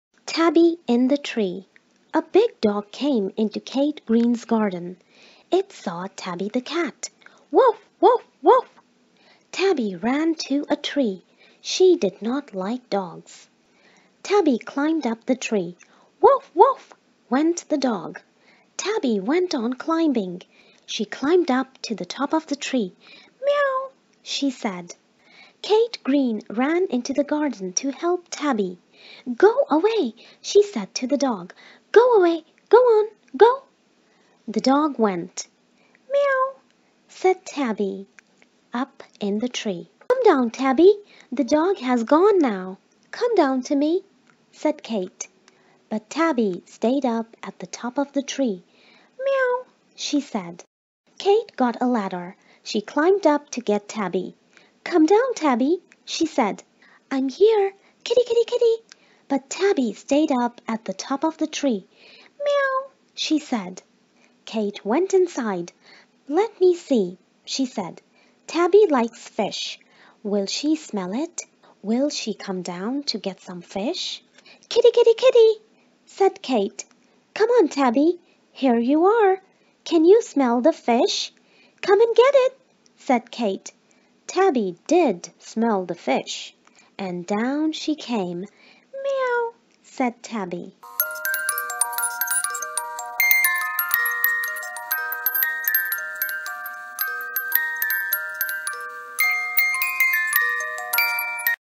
Tabby in the tree by by Beverley Randell & illustrated by Julian Bruere.
Tabby the cat gets scared by a dog and so she has climbed up a tree and now Kate Green can't get her down. Finally, she gets a brilliant idea to bring Tabby down.

Tabby in the tree by by Beverley Randell & illustrated by Julian Bruere. Publisher: Nelson Price Milburn Ltd PM Library